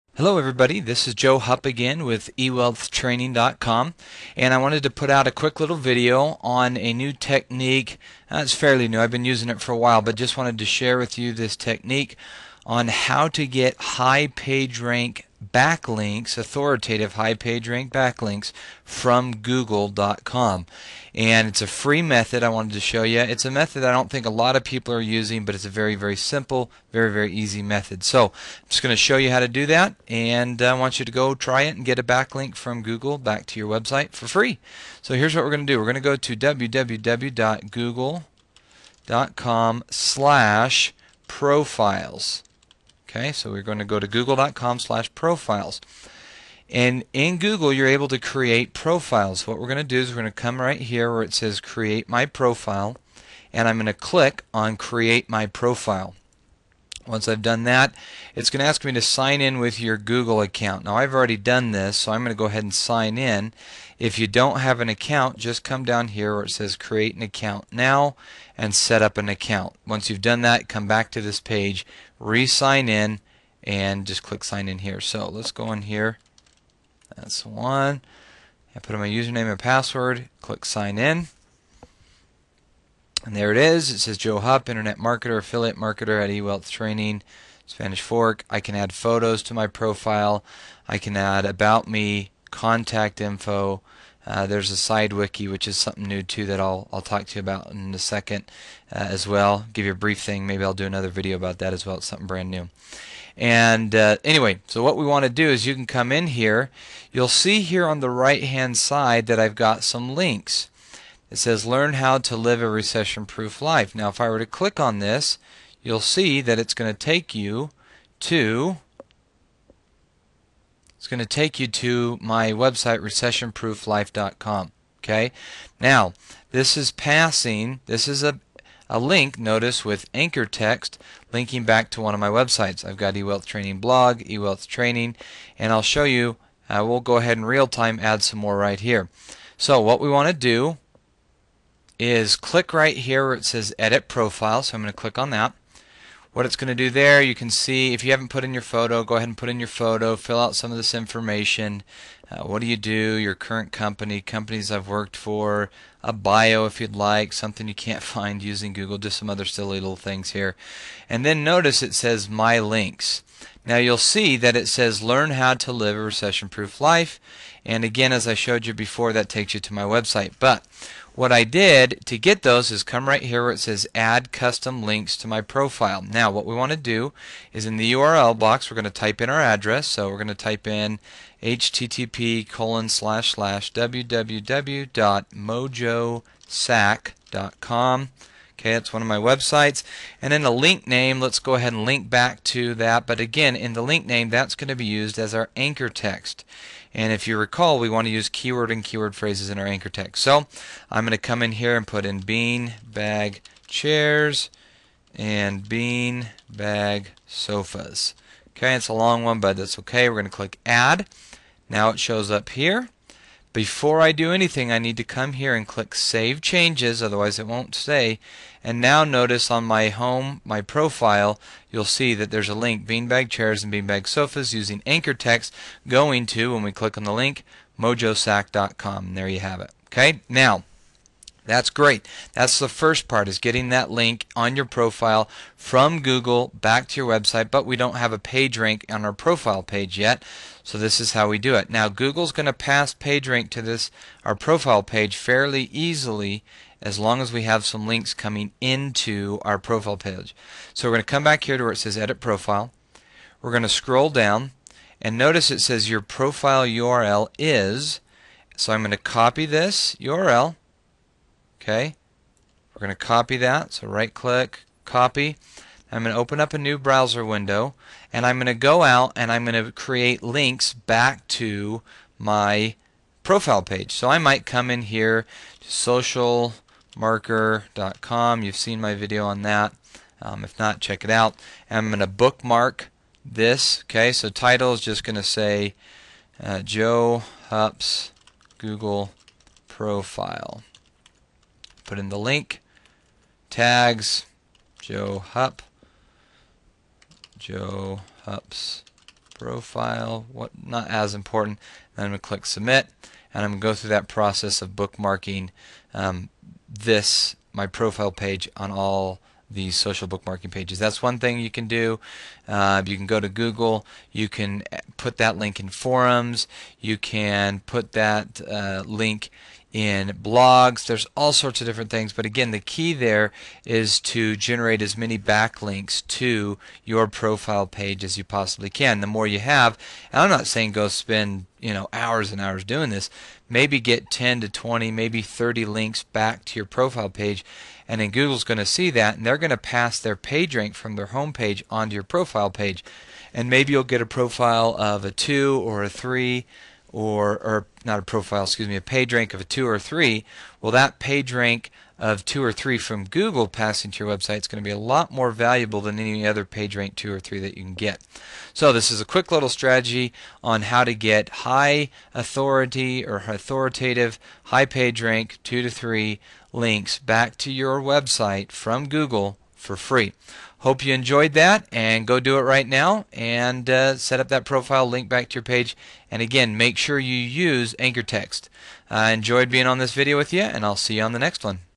How to get High Page Rank Baclinks from Google for FREE
Learn the secret to getting High PR Backlinks from Google for FREE using Google Profiles